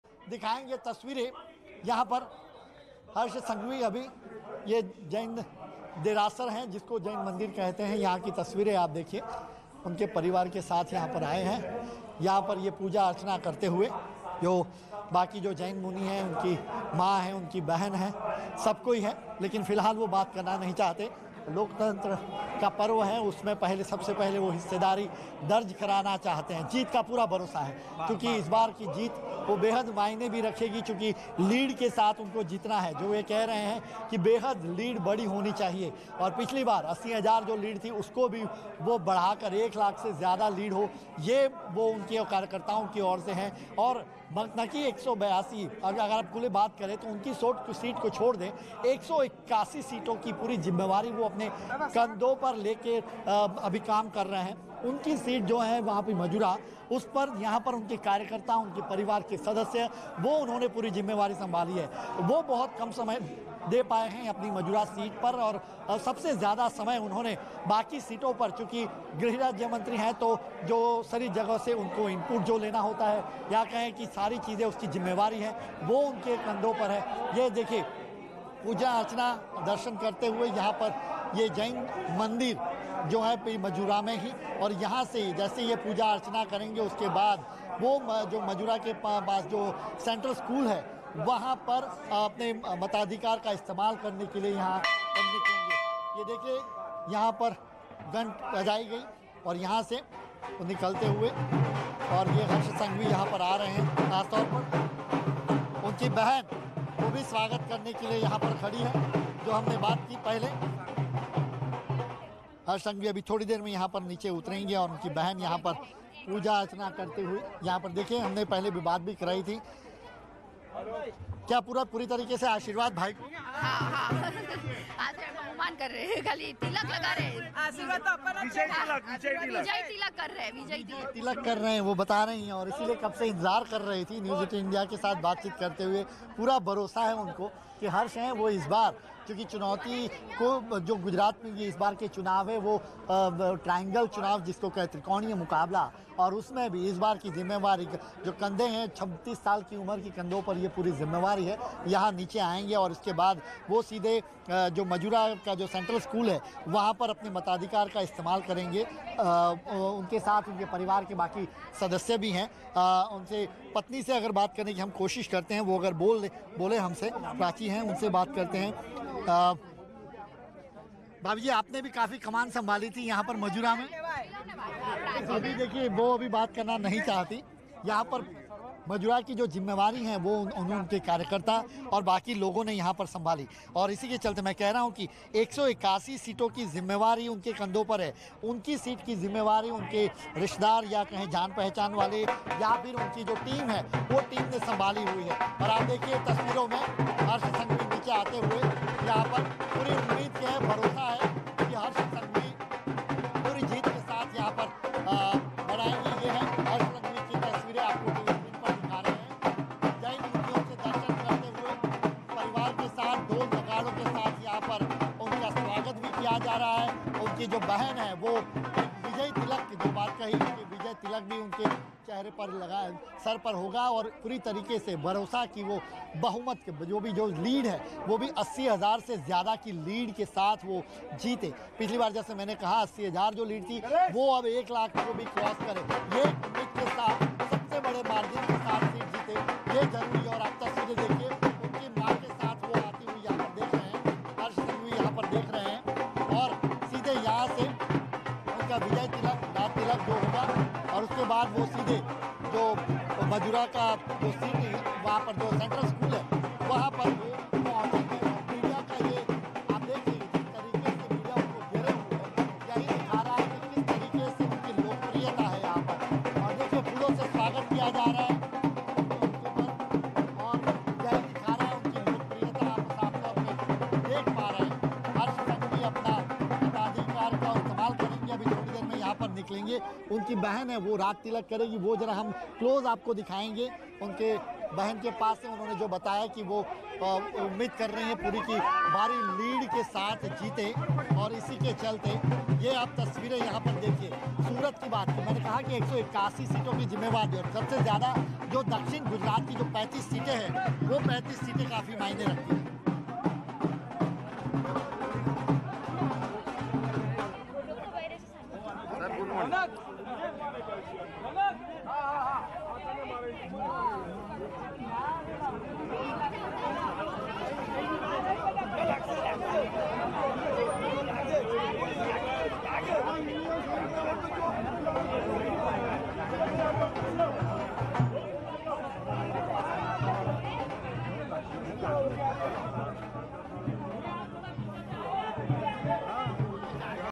Gujarat Election 2022:मतदान करने से पहले मंदिर पहुंचे गृह मंत्री Harsh Sanghavi | Phase1 |Hindi News
Gujarat Election 2022 : मतदान करने से पहले गुजरात के गृह मंत्री Harsh Sanghavi अपने परिवार सहित मंदिर पहुंचे .. मंदिर पहुंचकर उन्होंने पूजा अर्चना की .